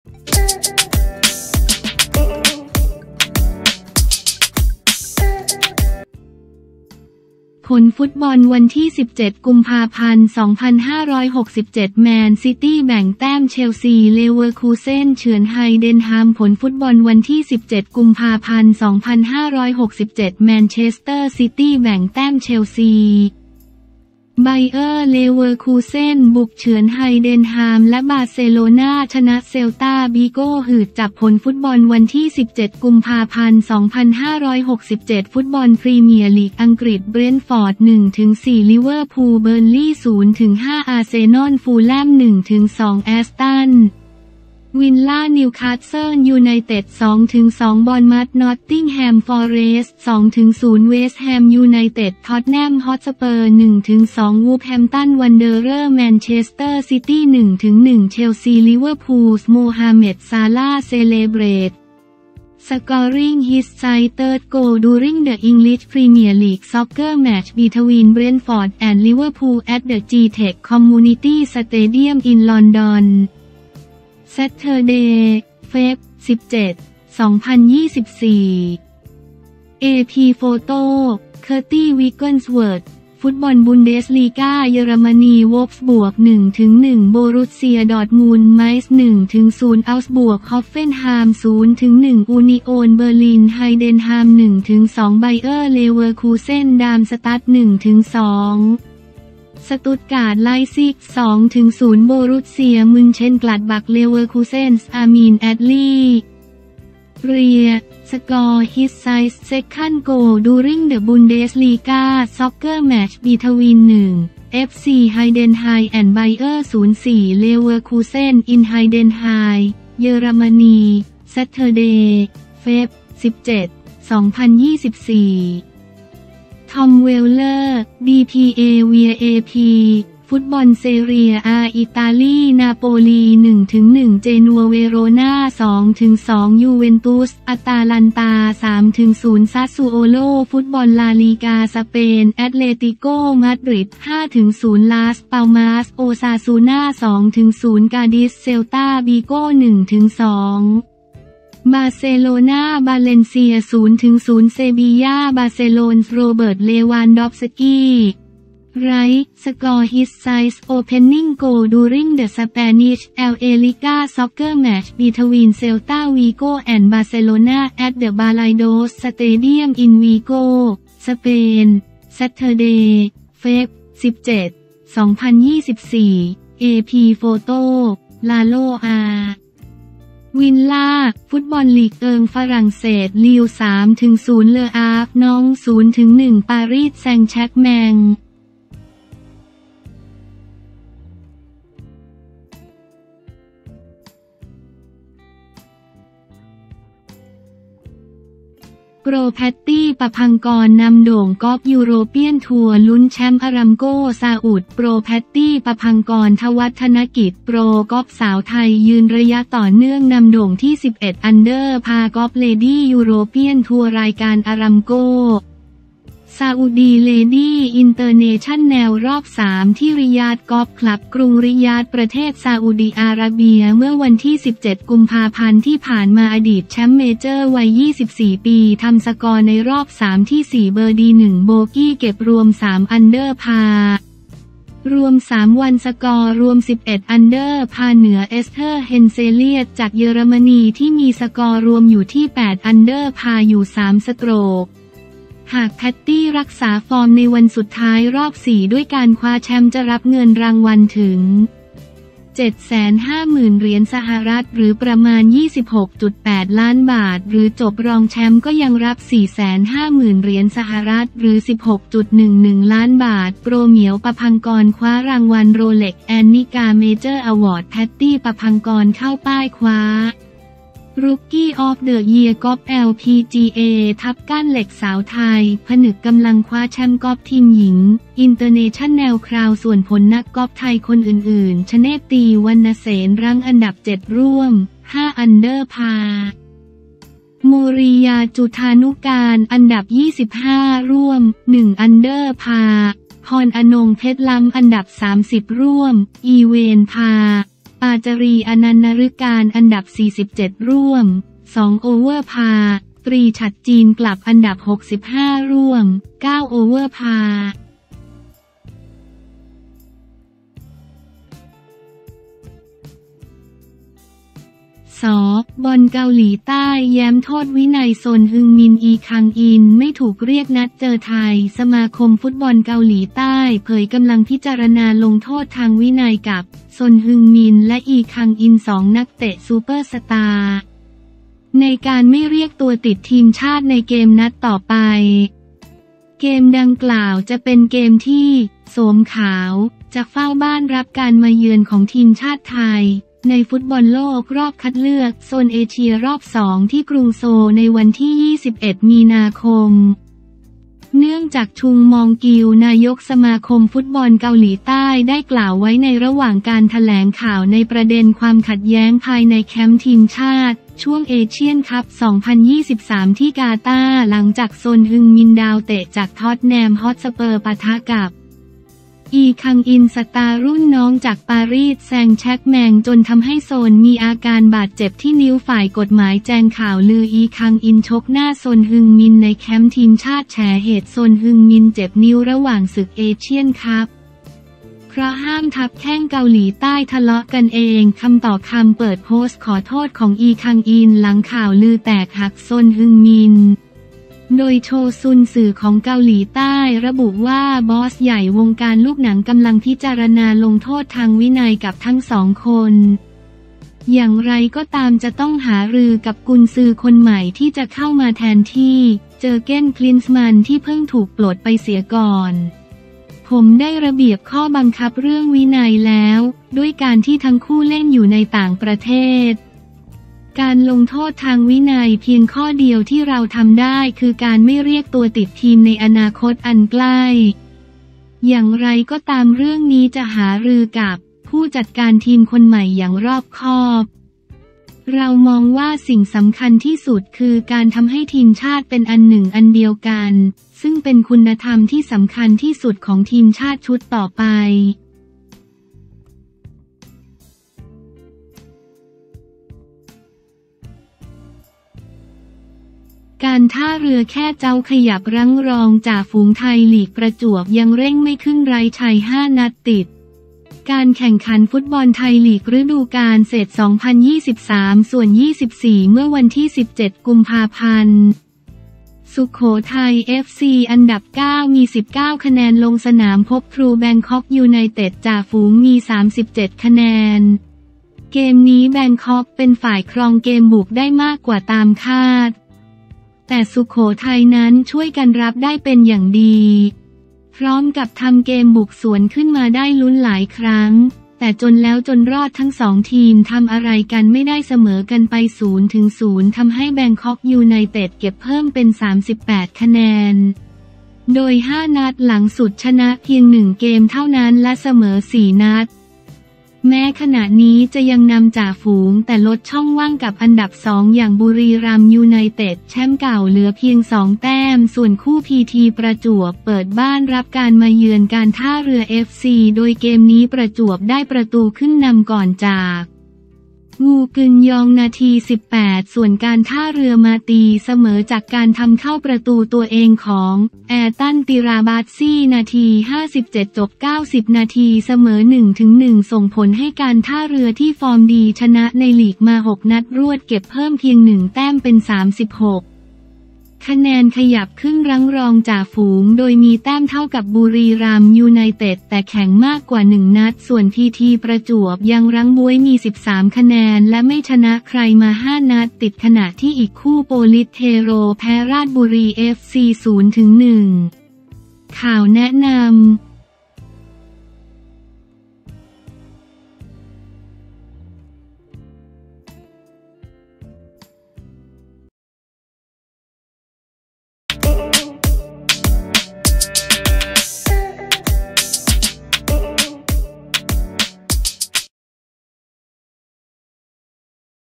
ผลฟุตบอล 17 ก พ 2567 แมนฯ ซิตี้ แบ่งแต้ม เชลซี เลเวอร์คูเซน เฉือน ไฮเดนไฮม์
ผลฟุตบอล 17 ก.พ. 2567 แมนเชสเตอร์ ซิตี้ แบ่งแต้ม เชลซี, ไบเออร์ เลเวอร์คูเซน บุกเฉือน ไฮเดนไฮม์ และบาร์เซโลนา ชนะ เซลตา บีโก หืดจับ

ผลฟุตบอล 17 กุมภาพันธ์ 2567
ฟุตบอลพรีเมียร์ลีก อังกฤษ

เบรนต์ฟอร์ด 1-4 ลิเวอร์พูล

เบิร์นลีย์ 0-5 อาร์เซนอล
ฟูแลม 1-2 แอสตัน วิลลา

นิวคาสเซิล ยูไนเต็ด 2-2 บอร์นมัธ

น็อตติงแฮม ฟอเรสต์ 2-0 เวสต์แฮม ยูไนเต็ด

ท็อตแนม ฮอตสเปอร์ 1-2 วูล์ฟแฮมป์ตัน วันเดอเรอร์ส

แมนเชสเตอร์ ซิตี้ 1-1 เชลซี
Liverpool’s Mohamed Salah celebrates scoring his sides third goal during the English Premier League soccer match between Brentford and Liverpool at the Gtech Community Stadium in London, Saturday, Feb. 17, 2024. (AP Photo/Kirsty Wigglesworth)
ฟุตบอลบุนเดสลีกา เยอรมนี

โวล์ฟสบวร์ก 1-1 โบรุสเซีย ดอร์ตมุนด์

ไมนซ์ 1-0 เอาก์สบวร์ก

ฮอฟเฟนไฮม์ 0-1 อูนิโอน เบอร์ลิน

ไฮเดนไฮม์ 1-2 ไบเออร์ เลเวอร์คูเซน

ดาร์มสตัดต์ 1-2 สตุตการ์ต

ไลป์ซิก 2-0 โบรุสเซีย มึนเชนกลัดบัก
Leverkusen’s Amine Adli, rear, scores his side’s second goal during the Bundesliga soccer match between 1. FC Heidenheim and Bayer 04 Leverkusen in Heidenheim, Germany, Saturday, Feb. 17, 2024. (Tom Weller/dpa via AP)
ฟุตบอลเซเรีย อา อิตาลี
นาโปลี 1-1 เจนัว

เวโรนา 2-2 ยูเวนตุส

อตาลันตา 3-0 ซาสซูโอโล

ฟุตบอลลา ลีกา สเปน

แอตเลติโก มาดริด 5-0 ลาส ปัลมาส

โอซาซูนา 2-0 กาดิซ

เซลตา บีโก 1-2 บาร์เซโลนา

บาเลนเซีย 0-0 เซบียา
Barcelona’s Robert Lewandowski, right, scores his side’s opening goal during the Spanish La Liga soccer match between Celta Vigo and Barcelona at the Balaidos stadium in Vigo, Spain, Saturday, Feb. 17, 2024. (AP Photo/Lalo R. Villar)

ฟุตบอลลีกเอิง ฝรั่งเศส

ลีลล์ 3-0 เลอ อาฟร์

น็องต์ส 0-1 ปารีส แซงต์ แชร์กแมง